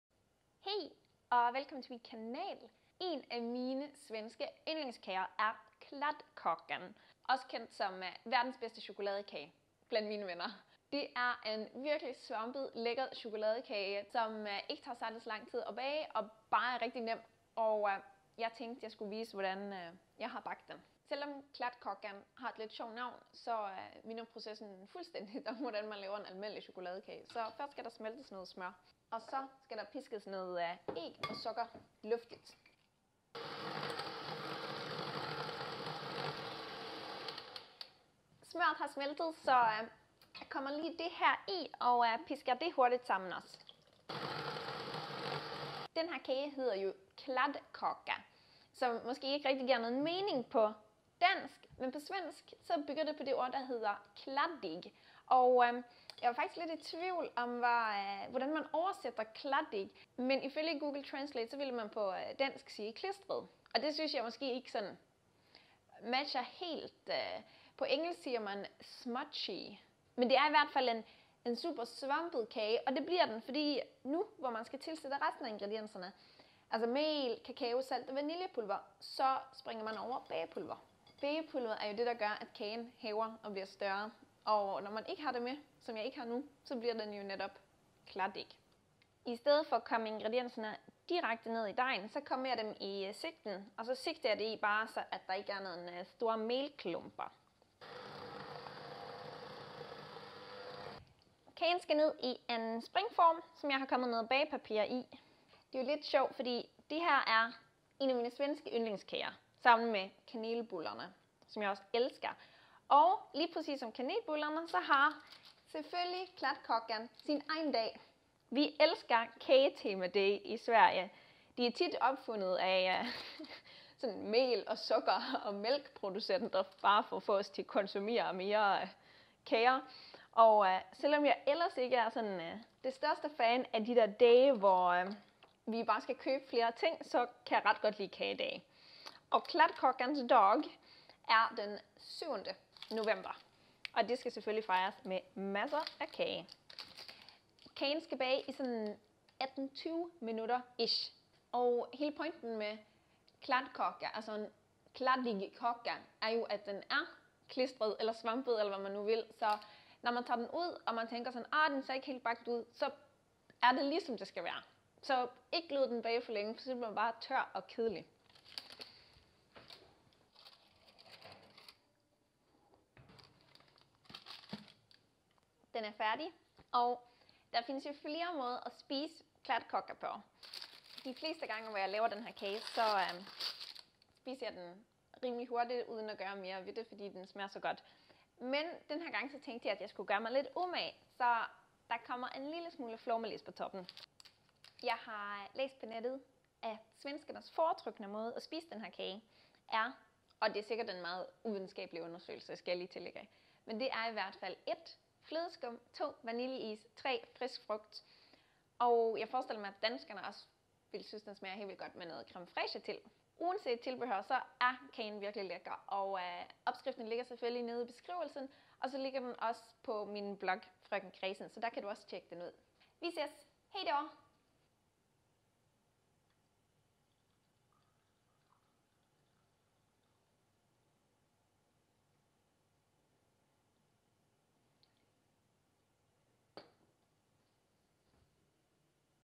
Opskrift: Chokoladekage | Svensk kladdkaka
Den her svenske chokoladekage - kladdkakan - er den bedste og hurtigste chokoladekage, jeg kender. Det tager kun 30 minutter at bage, og den er ikke specielt svær.

★ OPSKRIFT: Chokoladekage/Kladdkaka

100 g smør
2 æg
3 dl sukker
2,5 dl mel
4 spsk. kakao
1 nip vaniljepulver
1 nip salt

Smelt smørret.
Pisk æg og sukker luftigt.
Vend i smørret i dejen.
Kom mel, kakao, vaniljepulver og salt i en sigte.
Drys i kagen, og pisk sammen.
Kom kagen i en springform (cirka 25 cm i diameter) med bagepapir.
Bag 18-20 minutter ved 200 grader.

★ PYNT KAGEN PÆNT
I det her blogindlæg viser jeg, hvordan du kan pynte kagen på tre forskellige måder:

★ FLERE CHOKOLADEKAGER:
Her kan du se de andre chokoladekager, jeg har på min blog: